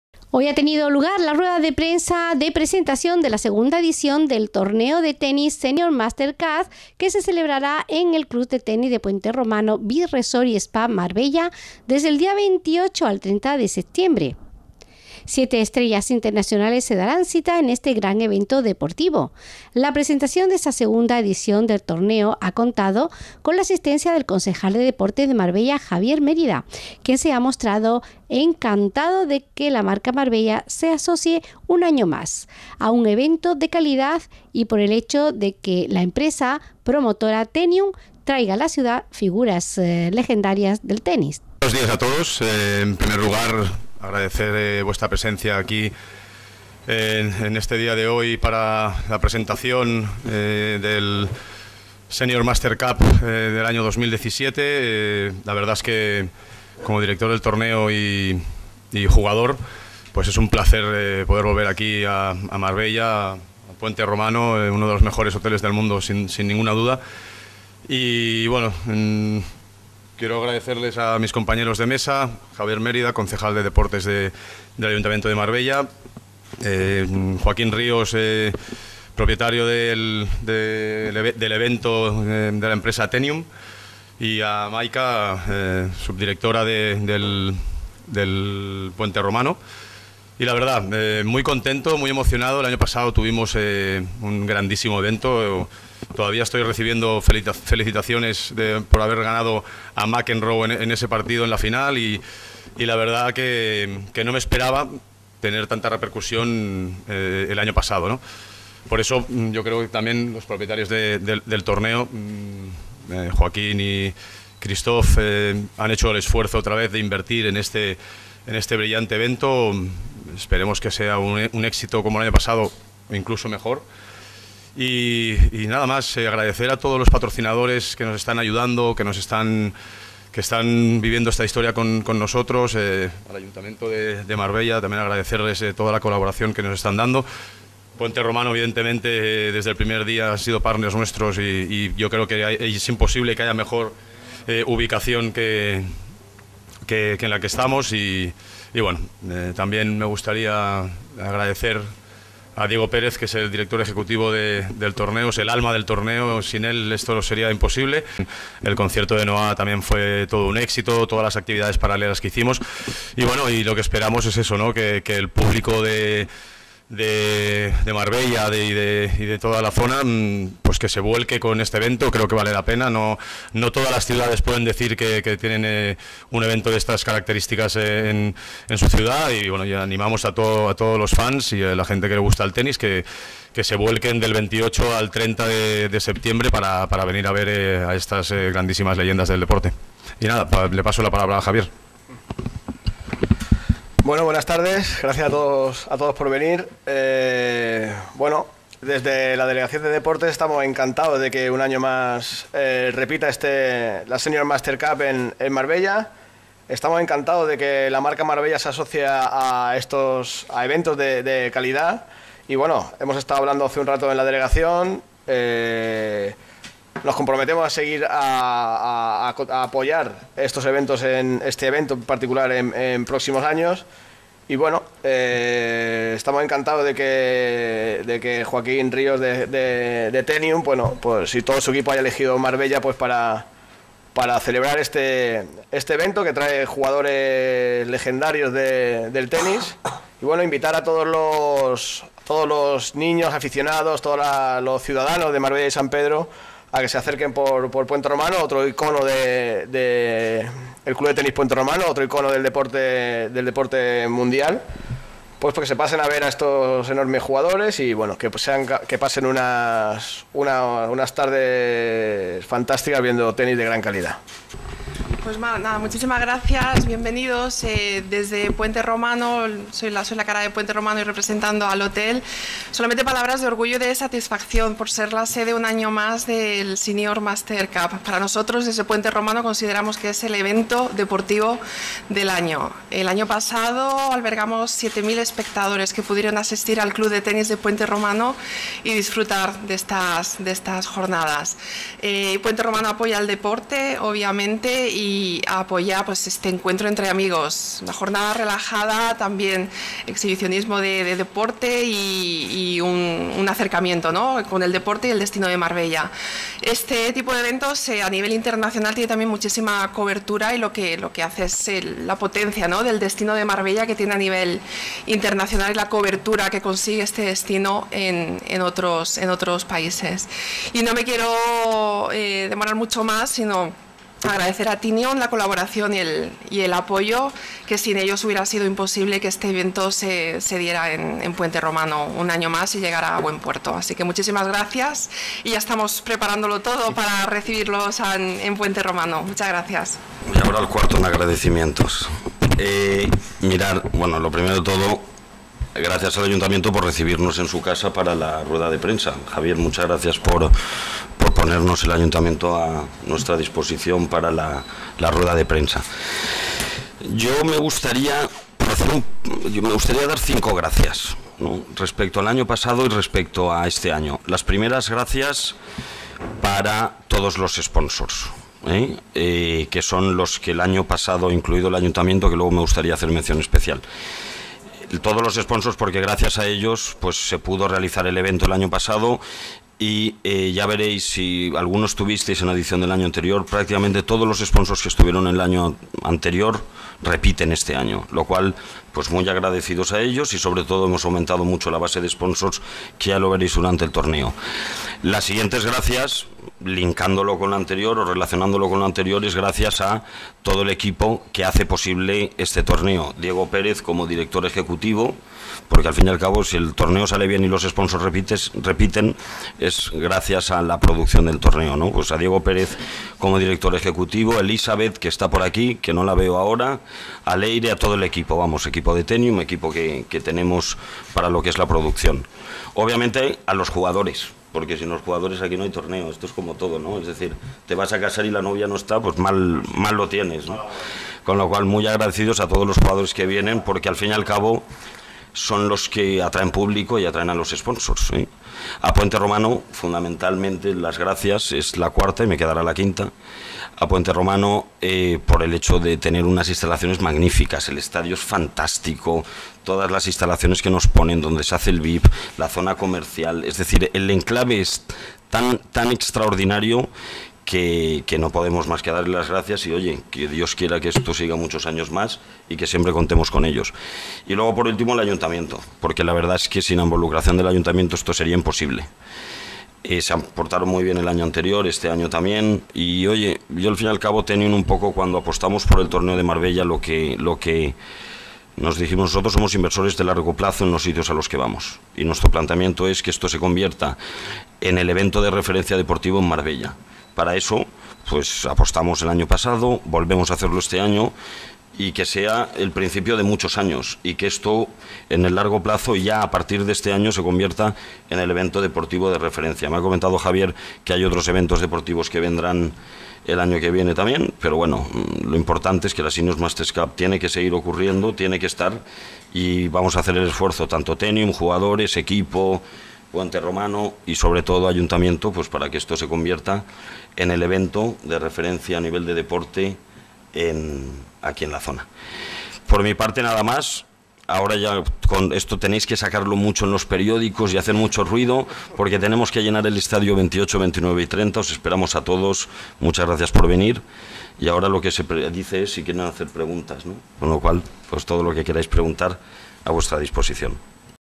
PRESENTACIÓN II SENIOR MASTERS CUP-MARBELLA
Marbella volverá a convertirse en referente del mundo del tenis a nivel mundial con el torneo Senior Masters Cup, que se celebrará del 28 al 30 de septiembre en el Club de Tenis de Puente Romano, con la participación de míticos jugadores como el estadounidense John MacEnroe, el francés Yannick Noah, el español Carlos Moyá y el sueco Mats Wilander.

 A las cuatro leyendas mencionadas, se sumarán el australiano Pat Cash (campeón en Wimbledon en 1987), el español Albert Costa (director del torneo en Marbella y ganador de la edición 2016) y el francés Sebastián Grosjean.

 El evento ha sido presentado hoy en el Ayuntamiento por el concejal de Deportes, Javier Mérida, el propio Albert Costa, la subdirectora del Hotel Puente Romano Beach Resort & Spa Marbella, Maica Huertas, y el socio y director ejecutivo de Tennium (empresa promotora del evento), Joaquín Ríos.

 El concejal de Deportes ha subrayado que “desde el Ayuntamiento estamos encantados por la celebración del torneo Senior Masters Cup un año más y por el hecho de que la marca Marbella se asocie a eventos de calidad, como en este caso” y ha añadido que por este motivo el Consistorio seguirá apoyándolo en próximas ediciones.

 Los partidos del torneo tenístico serán televisados por Teledeporte (TVE). El jueves día 28 de septiembre se disputará el de Grosjean contra Costa, a las 18.00 horas, y le seguirá el de Wilander contra Cash, a las 20.00 horas. Al día siguiente, el viernes 29 de septiembre, Carlos Moyá jugará con el vencedor del partido entre Wilander y Cash (18.00 horas) y John McEnroe con el que lo haga en el de Grosjean-Costa (20.00 horas). Ya el sábado día 30 de septiembre tendrá lugar a las 18.00 horas el partido de dobles de exhibición con Noah y a las 20.00 horas la final.

 La edición 2016 de la Senior Masters Cup contó con una asistencia de 7.000 espectadores durante las tres jornadas y con 500.000 televidentes totales. Además de 137.000 personas alcanzadas en Facebook y 275.000 impresiones en dicha red social y 64.500 en Twitter, “cifras que describen el éxito del torneo de veteranos más importante de la temporada”, según Albert Costa.

 Por su parte, Maica Huertas y Joaquín Ríos han agradecido a los sponsors, Ayuntamiento, jugadores y al equipo que hace posible el torneo su involucración con el mismo y han animado a ciudadanos y visitantes a disfrutar de este evento ya consolidado.